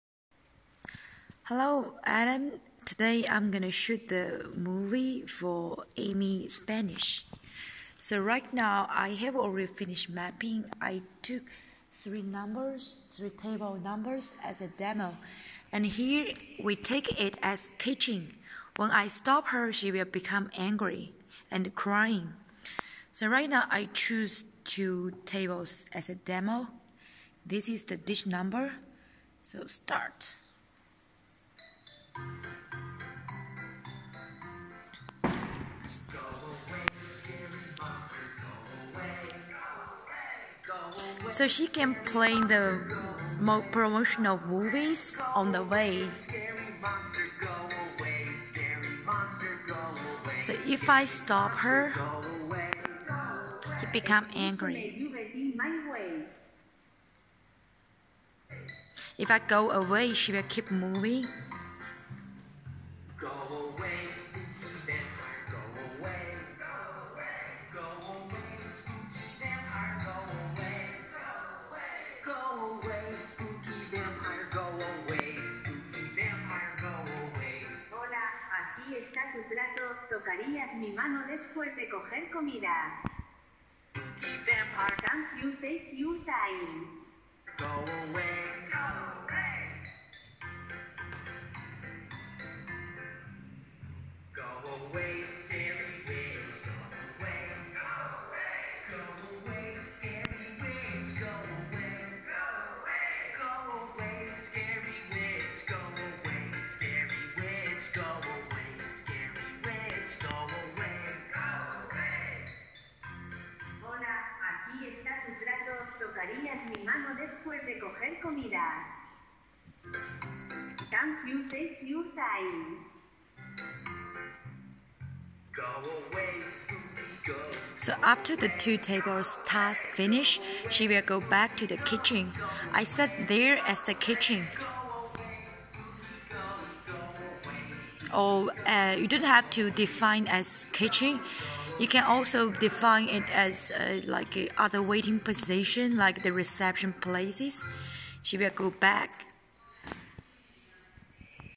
¡Robot Waiter Amy empieza a hablar español ahora!
El camarero robot más avanzado con precios asequibles para restaurantes modernos.